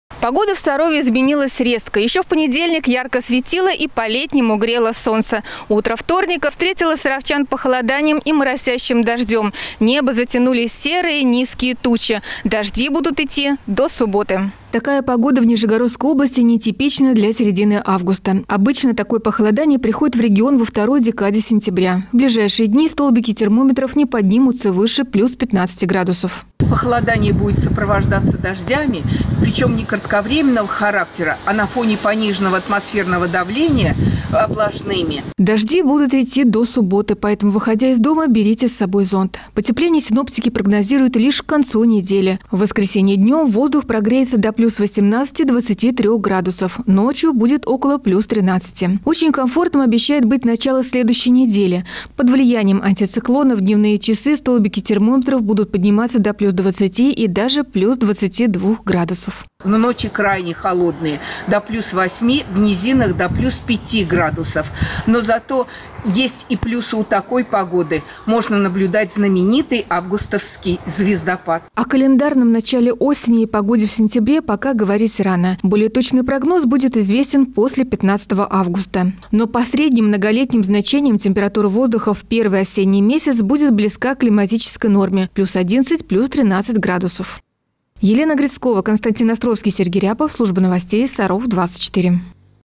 Каким будет август?
На европейскую часть территории России раньше времени пришла осенняя погода. Стоит ли ждать возвращения жарких дней?